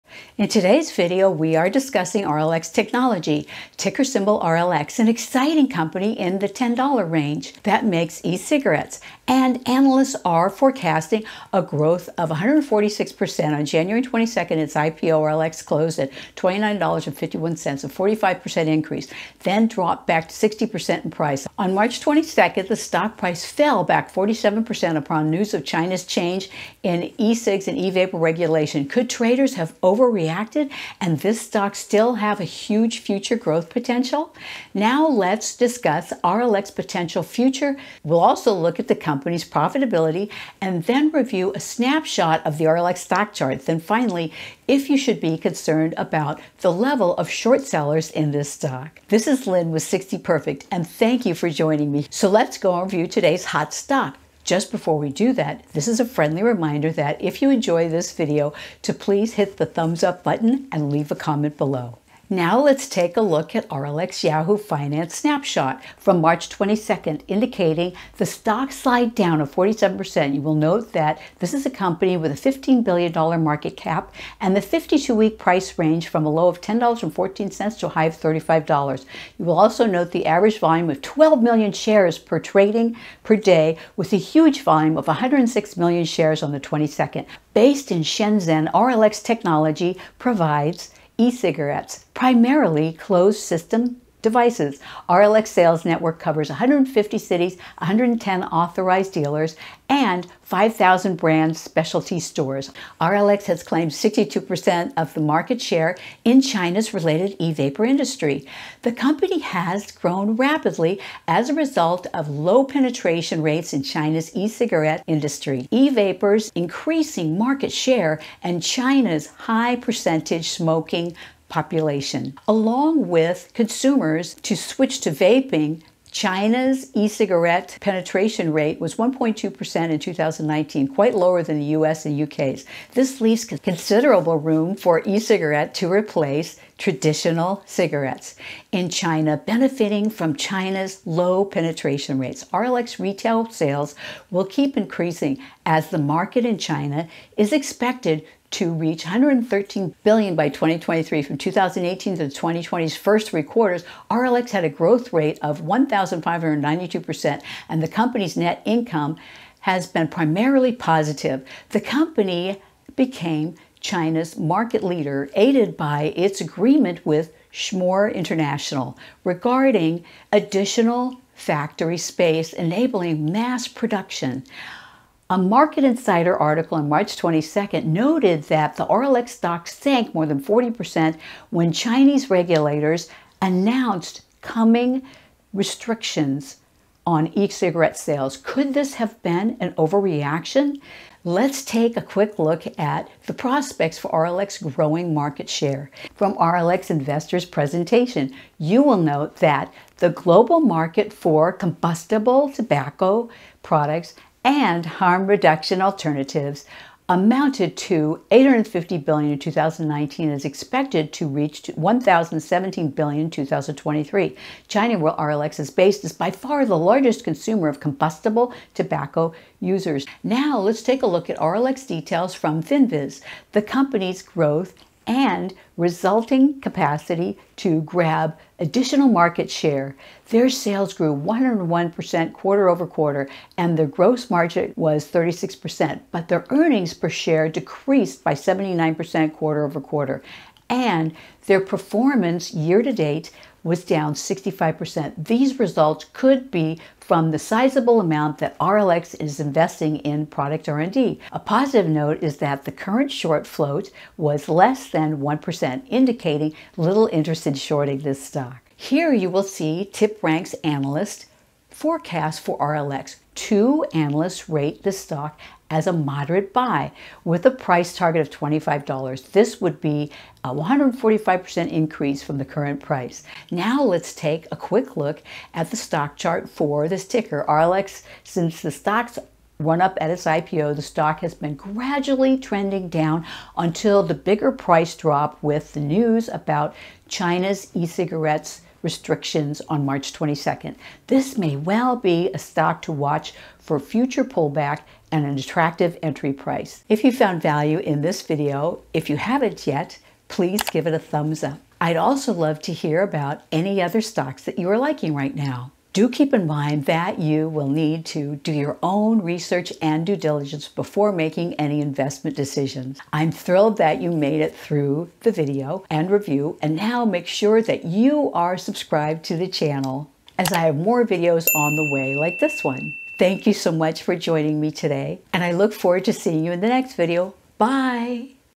💥RLX Stock Forecast +145% Analysts Buy!
In Today’s video we are discussing, RLX Technology, ticker symbol, RLX. An exciting company in the $10 range, that makes e-cigarettes, and analysts are forecasting a growth for RLX of 146%.  On January 22nd, at its IPO, RLX closed at $29.51, a 145% increase in one day, then later dropped back 60% in price. On March 22nd, the stock price fell back 47% upon news of China’s change in e-cigs/e-vapor regulation. Could traders have overreacted, and this stock still have a huge future growth potential?

We will discuss RLX’ potential future, China’s and the global e-Cig market. We’ll also look at the company’s profitability and review a snapshot of the RLX stock chart, and the level of short sellers in this stock.

🕑Table of Contents:
0:00:00 RLX Technologies and what we will cover in this video
0:01:05 RLX' Yahoo Finance Snapshot: Market cap, 52 week trading range and Volume.
0:01:28 Based in China - this segment covers RLX' Market share and potential growth in China and globally.
0:02:55 Market Insider article about RLX stock sinking 45% on the 22nd. Market potential from RLX Technologies' Investors presentation.
0:03:43: Finviz overview for RLX - performance year-to-date and quarter-over quarter. Short float.
0:04:28 TipRanks' Analysts forecast for RLX Technologies.
0:04:38 RLX Chart analysis.